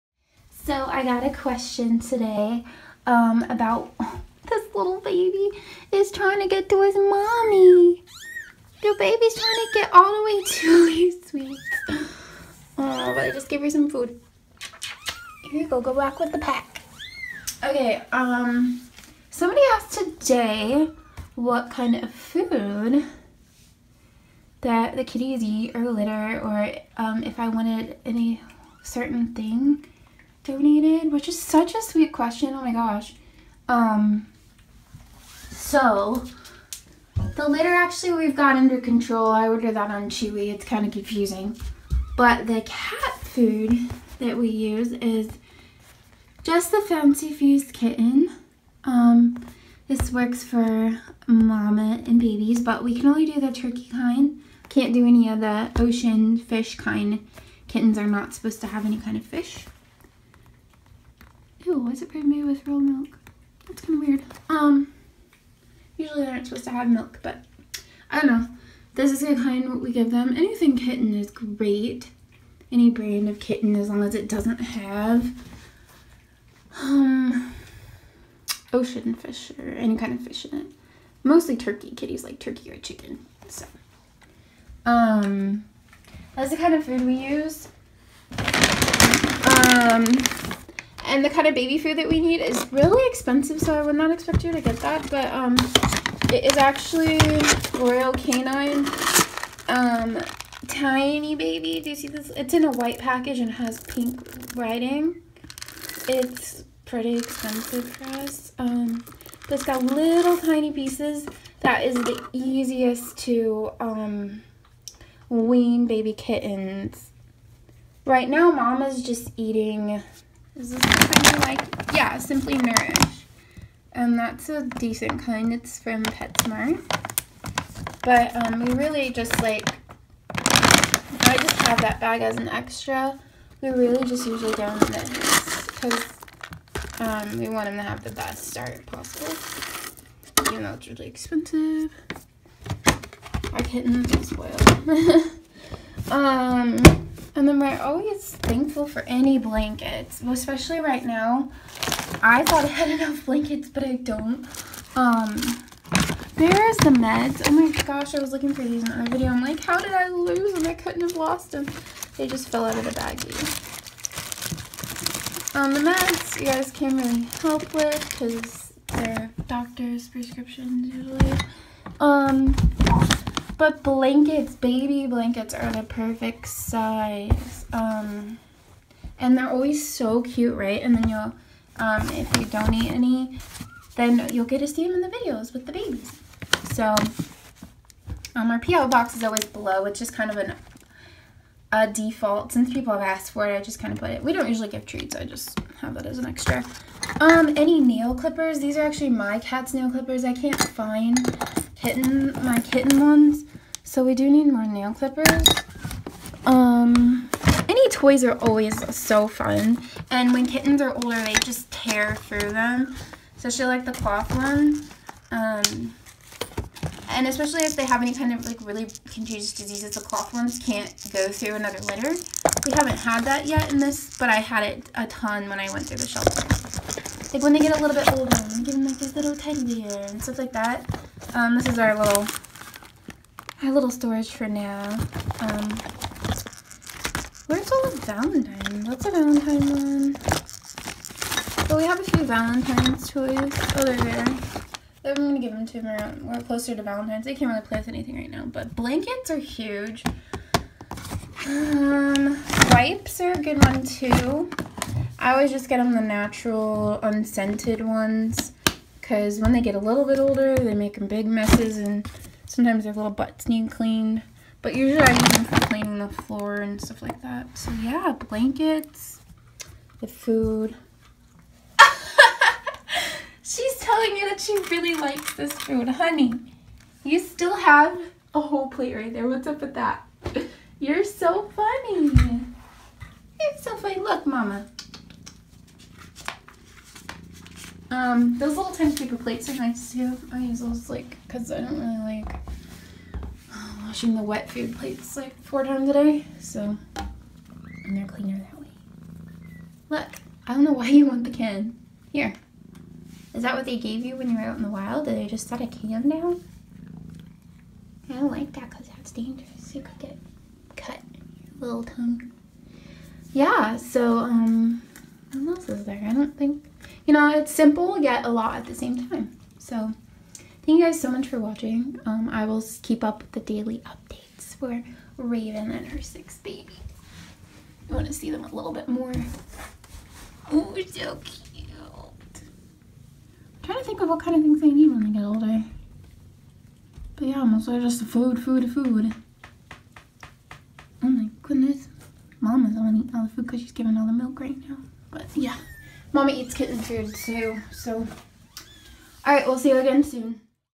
Items Momma Cat and Her Babies Need! New Only!
It has been suggested to do an amazon or chewy wishlist I will work on that! I can't believe you all are so kind to want to give the kittens stuff thank you so much!!